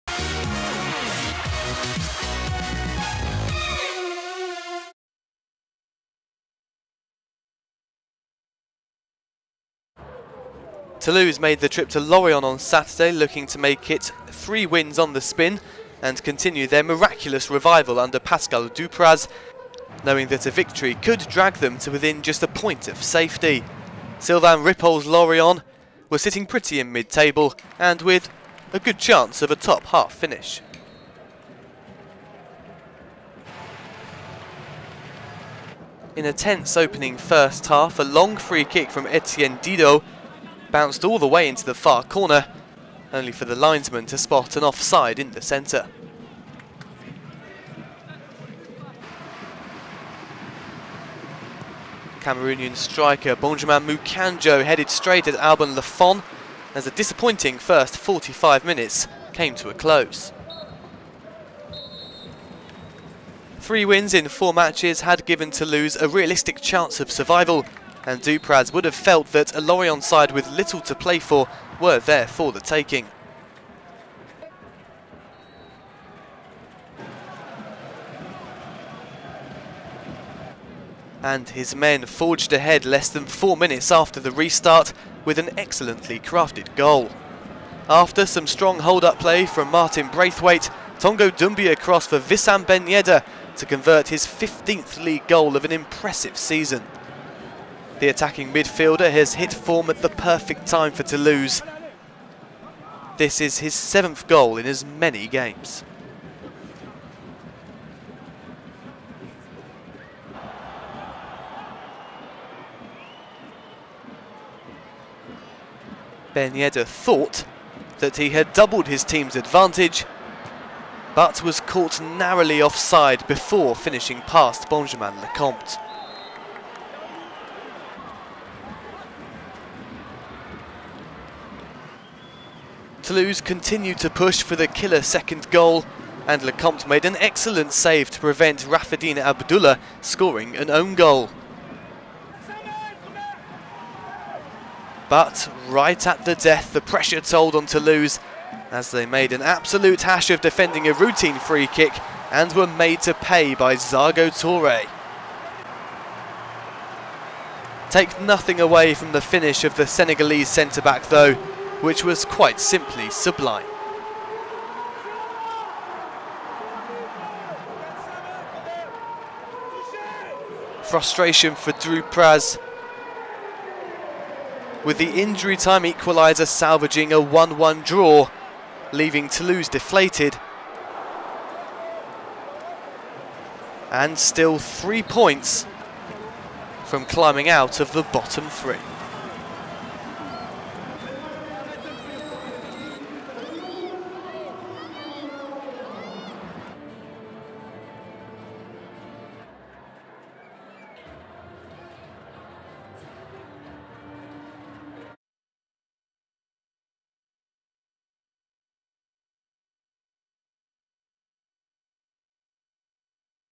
FC Lorient - Toulouse FC (1-1) - Highlights - (FCL - TFC) / 2015-16
FC Lorient vs Toulouse FC (1 - 1) highlights. The best actions and goals of FC Lorient vs Toulouse FC in video.

Stade Yves Allainmat - Le Moustoir - Sat, 16 Apr 2016

Goals : Wissam BEN YEDDER (49' - Toulouse FC) / Zargo TOURE (90' +1 - FC Lorient)

FC Lorient Team line-up : 5 - Zargo TOURE / 7 - Didier NDONG / 12 - Benjamin MOUKANDJO / 13 - Raffidine ABDULLAH / 14 - Raphaël GUERREIRO / 19 - Romain PHILIPPOTEAUX / 22 - Benjamin JEANNOT / 25 - Lamine GASSAMA / 26 - Lindsay ROSE / 28 - Maxime BARTHELME / 40 - Benjamin LECOMTE (c)

Toulouse FC Team line-up : 3 - Jean-Armel KANA-BIYIK / 4 - Tongo Hamed DOUMBIA / 8 - Etienne DIDOT / 9 - Martin BRAITHWAITE (c) / 10 - Wissam BEN YEDDER / 17 - Adrien REGATTIN / 20 - Steeve YAGO / 26 - Marcel TISSERAND / 27 - Alexis BLIN / 29 - Jacques François MOUBANDJE / 40 - Alban LAFONT